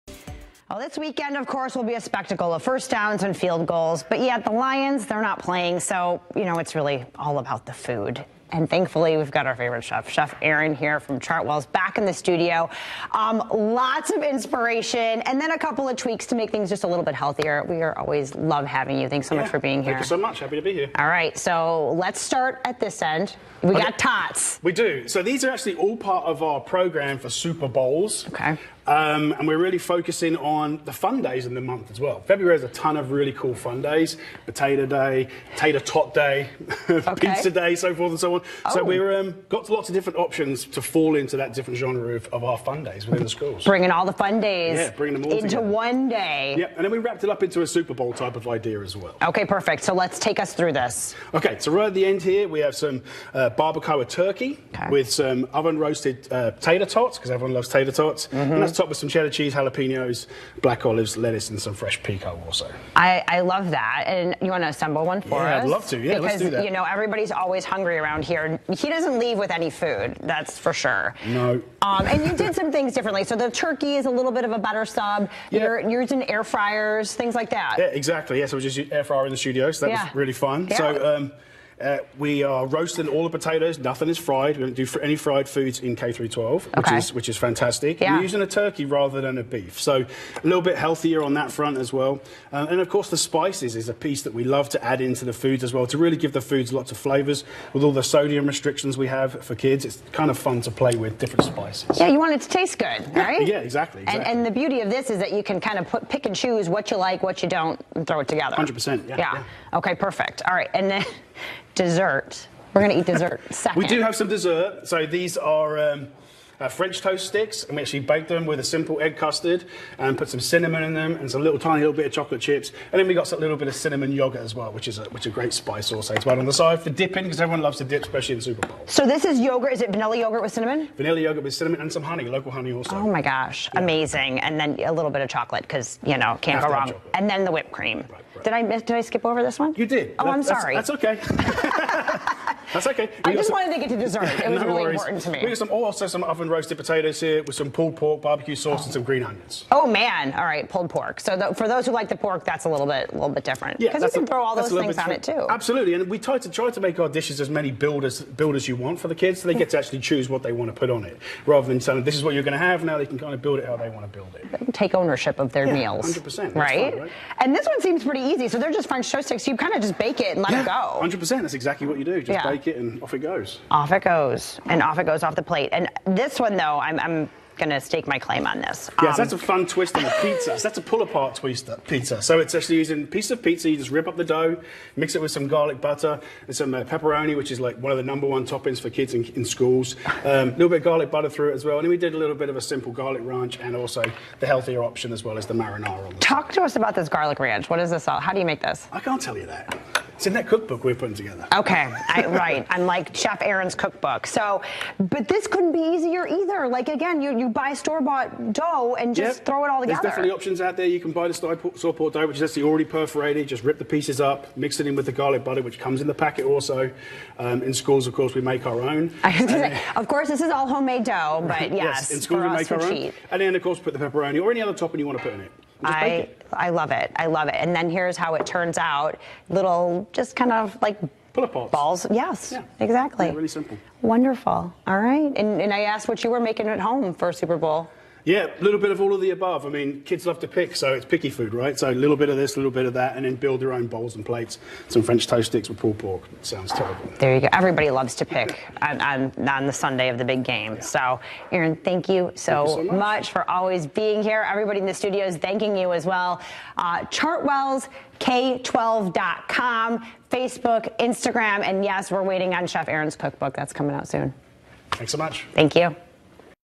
Chef Aaron shares recipes to cook up for your Big Game watch party
Chef Aaron from Chartwells K12 joined the Fox 17 Morning Mix studio to share some of his favorite game day meals, while also presenting some sneaky ways to put some healthier choices on the table.